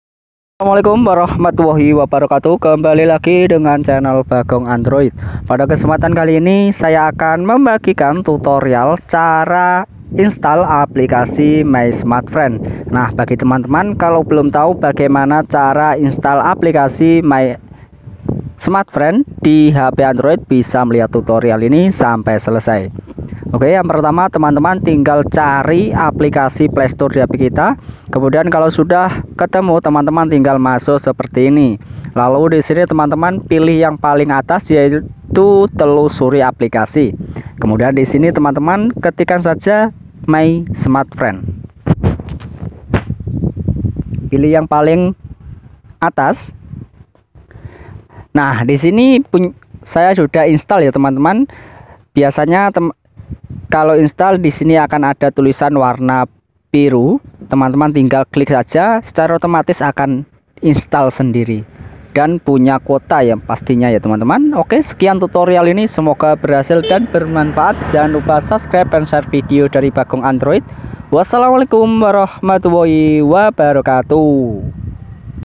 Cara Instal Aplikasi My Smartfren Terbaru
Halo teman-teman kembali lagi dengan channel Bagong Android Pada kesempatan kali ini saya akan membagikan tutorial Cara Instal Aplikasi My Smartfren Terbaru

Nah Cara Instal Aplikasi My Smartfren 2024 tonton terus video ini sampai selesai Agar kalian paham